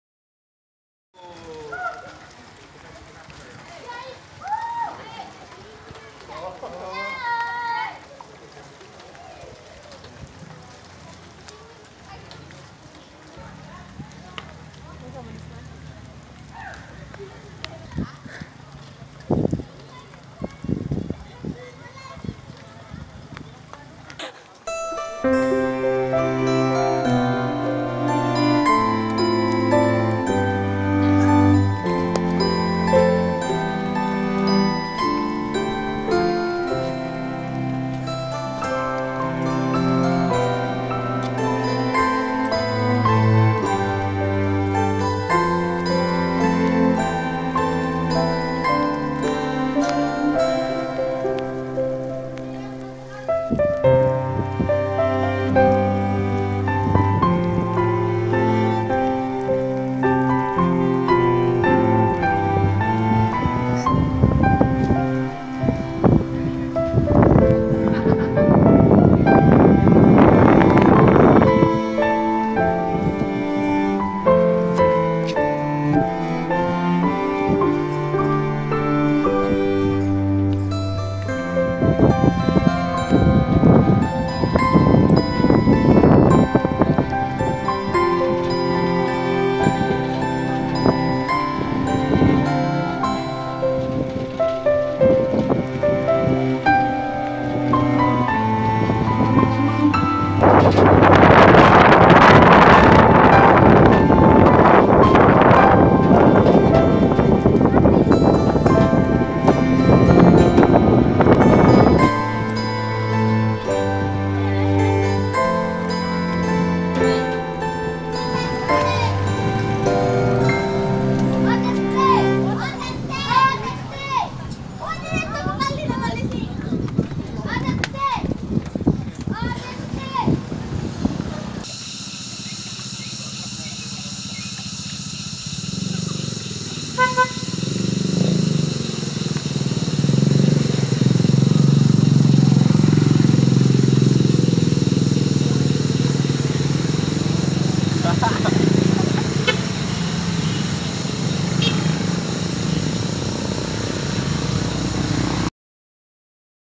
Sensasi Kabut di Agro wisata Pango pango
Agro Wisata Pango pango terletak di Kecamatan Makale Selatan, Tana Toraja. Disini pengunjung akan dimanjakan dengan keindahan alam nan sejuk.